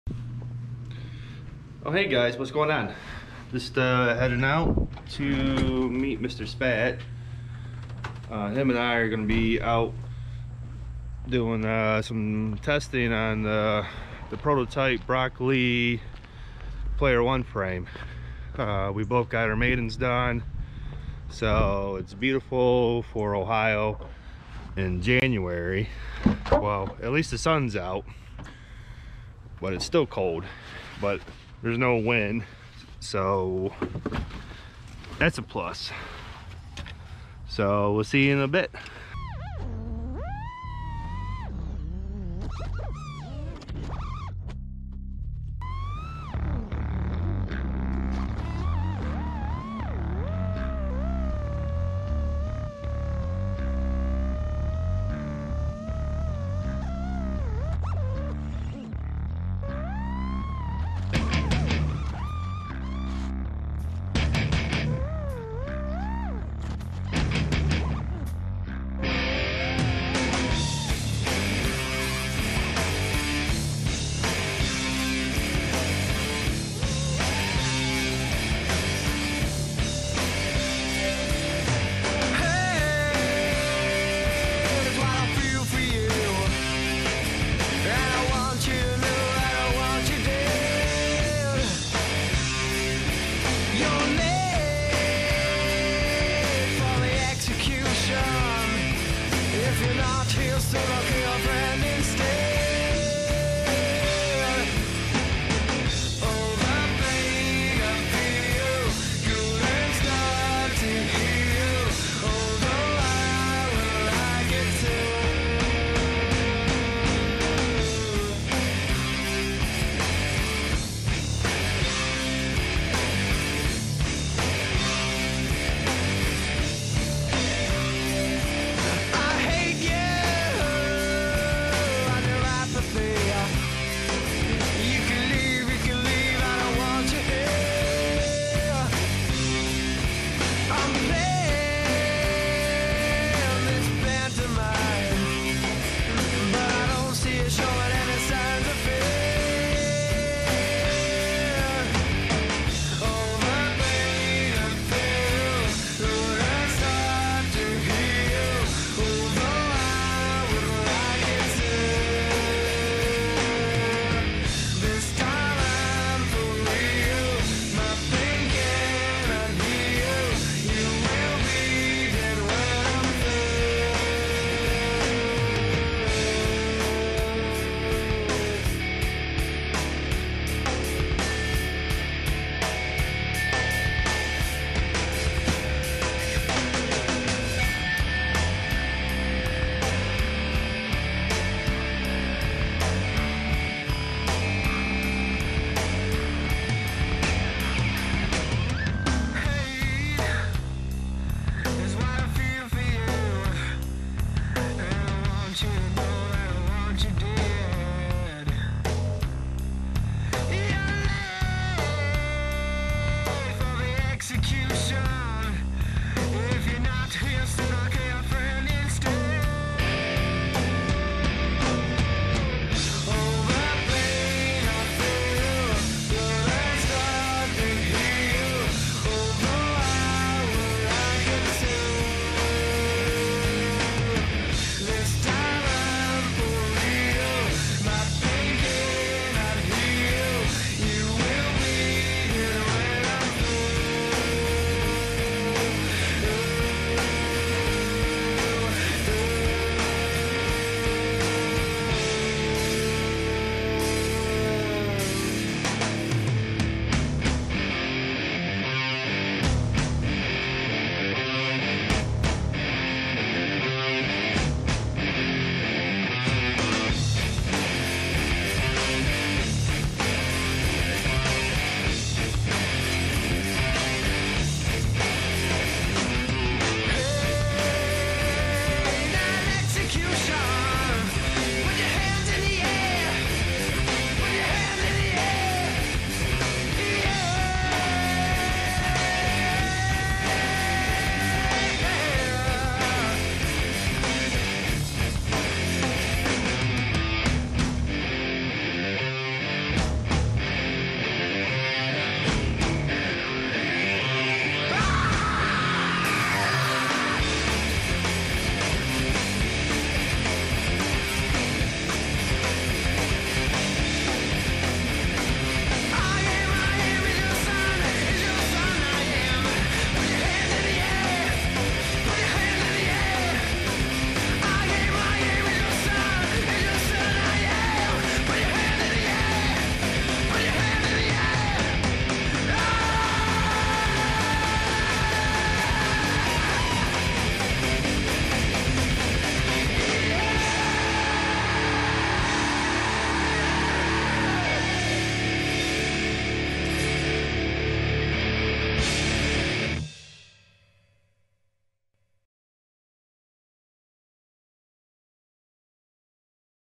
READY PLAYER 1 prototype freestyle player1 🎮🥦🥽
On a sunny and cold January afternoon. The player 1 was flown and crashed.  So far the testing is going great. It has a perfect balance on the roll and pitch axis. Holding up to crashes even in 30 degree weather. After the tweaks are done and then I will test the next version of changes. After that it will be released out into the wild. Stay tune for more updates and content as we continue down the test road.

I do not owe the rights to this music